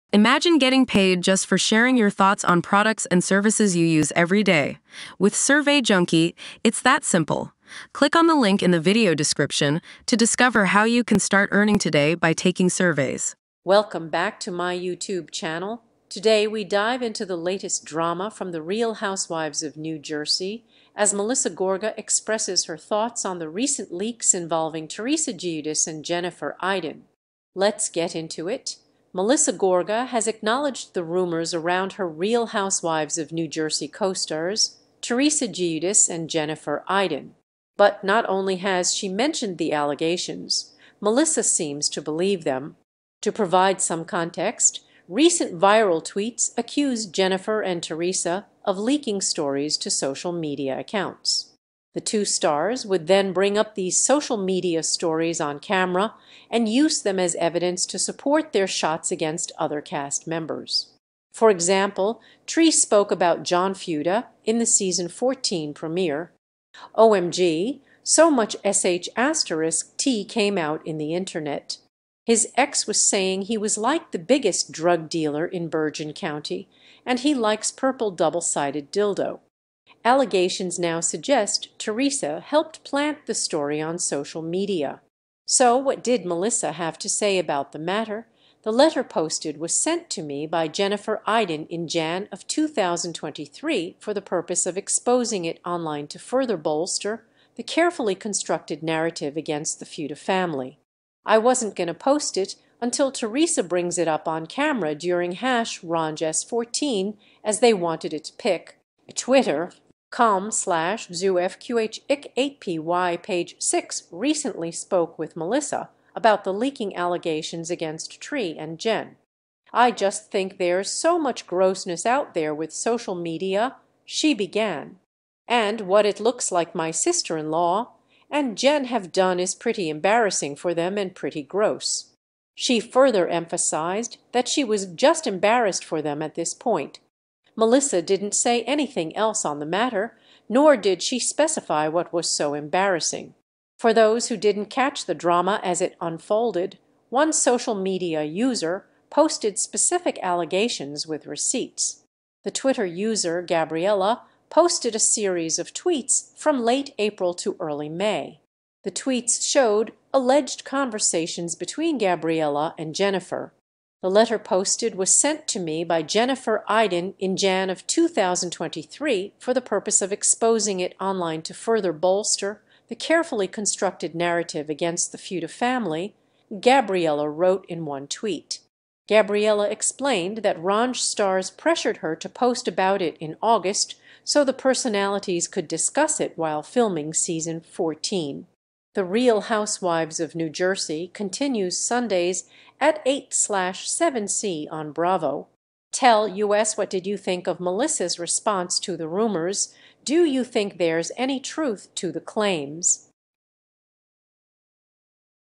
Melissa Gorga Speaks Out on RHONJ Leaks: Teresa Giudice and Jennifer Aydin Embarrassed
In this video, we discuss Melissa Gorga's reaction to recent leaks from the Real Housewives of New Jersey. Melissa finds the leaks involving Teresa Giudice and Jennifer Aydin to be embarrassing and shares her thoughts on the situation. Tune in to hear all the details and insights on this juicy RHONJ news!